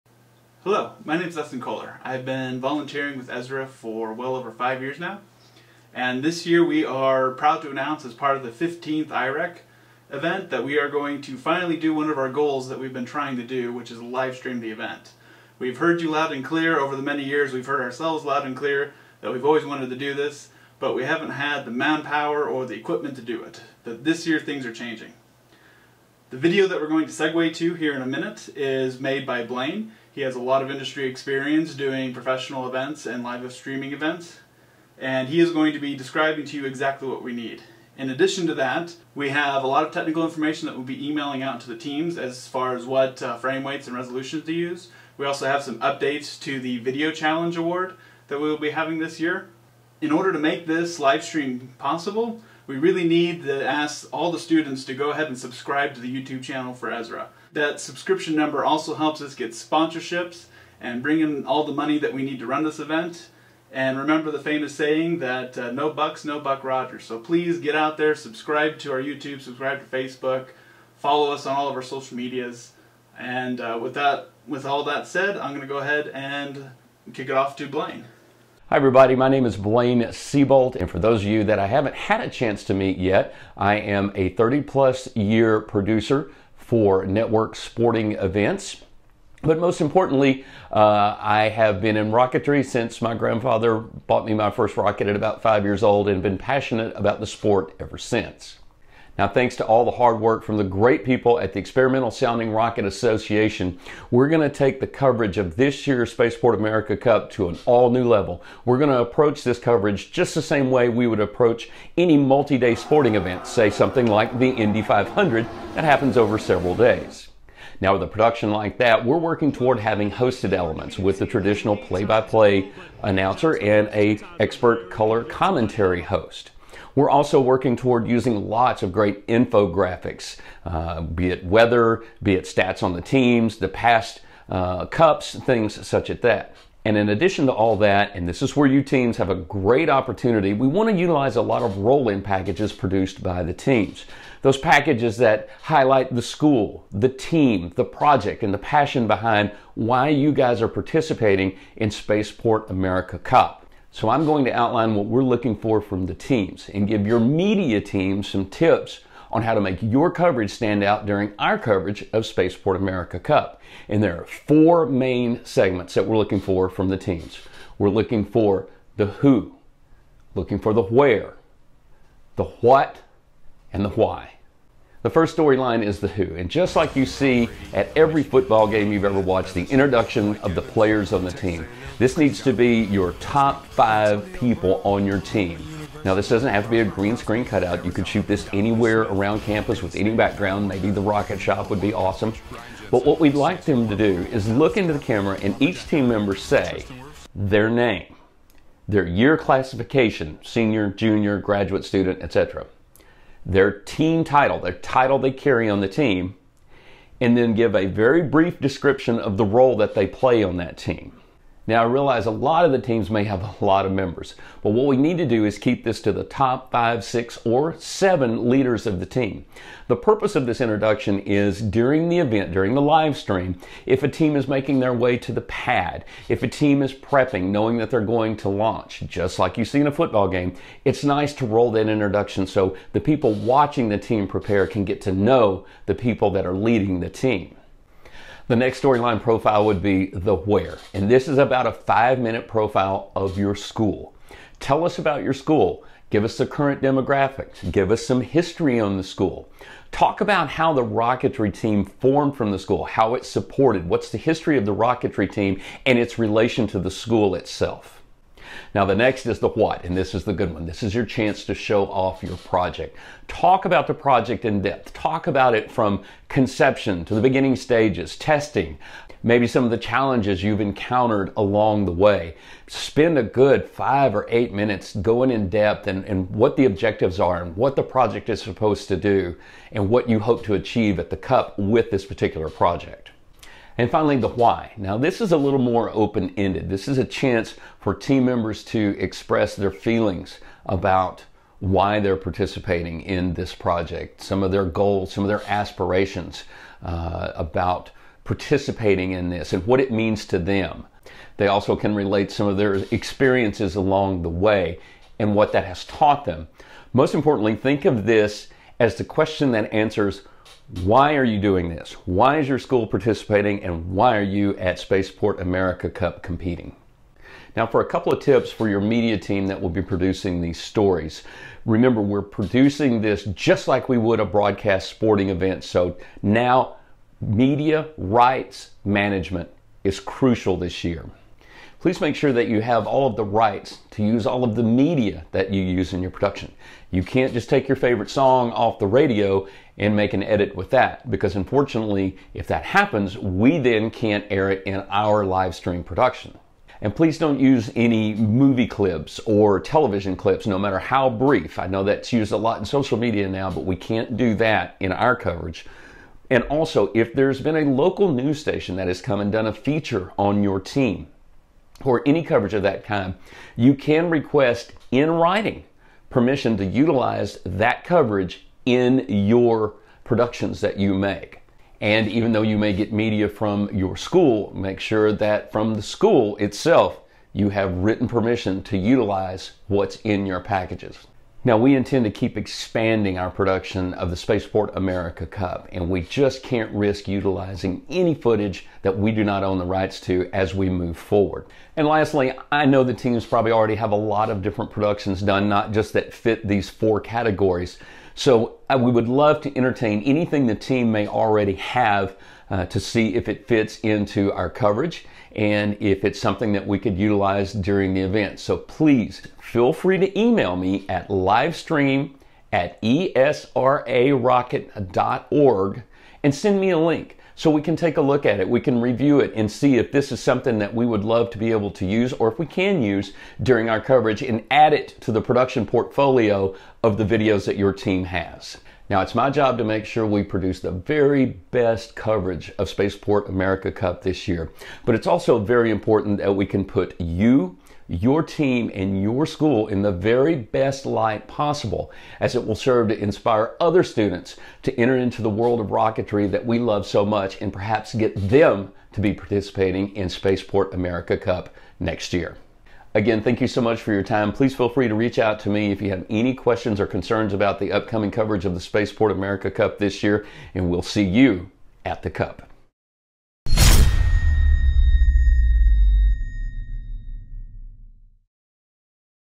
ESRA Live Stream Announcement
Proud to announce we will be live streaming this year's 15th IREC at the 2022 Spaceport America Cup. Teams should follow the instructions in the video to help pre-prepare content for this year's event.